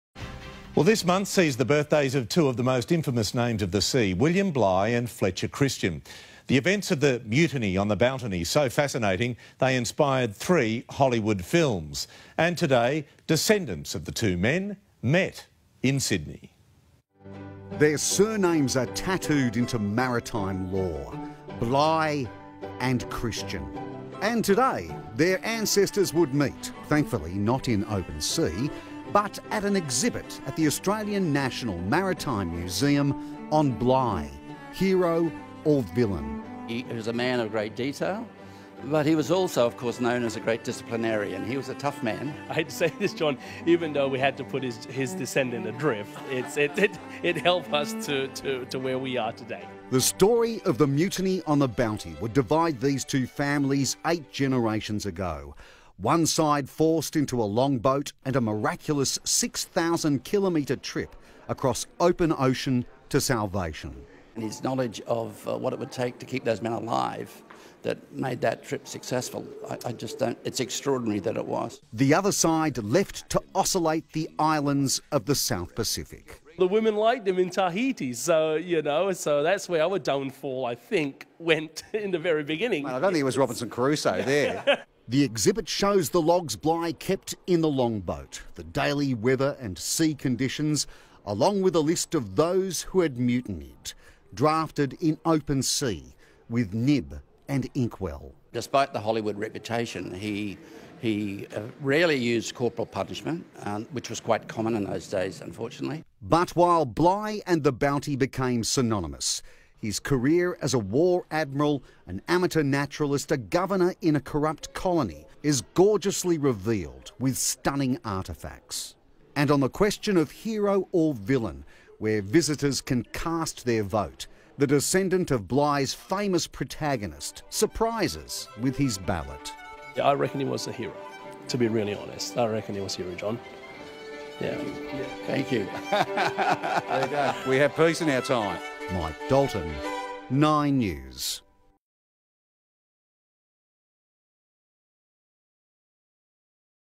Meet William Bligh and Fletcher Christian's Descendants
Descendants of William Bligh and Fletcher Christian meet up to have a fun discussion about their ancestors at our new exhibition Bligh: Hero or Villain? What did they decide...?🤔